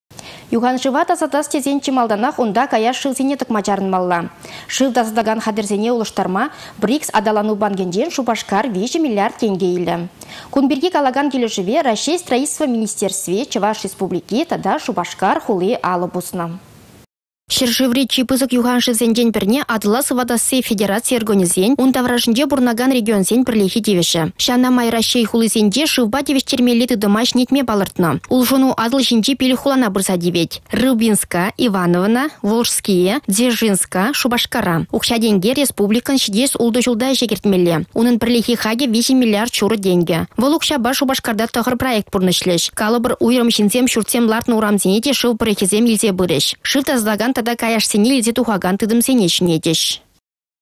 Юханшыва тасатас тесен чи малтанах унта каяш шывсене тăкма чарăнмалла.
Шыв тасатакан хатĕрсене улăштарма Брикс аталану банкĕнчен Шупашкар 3 миллиард тенкĕ илĕ. Кун пирки калакан килĕшĕве Раççей строительство министерстви, Чăваш Республики тата Шупашкар хули алă пуснă.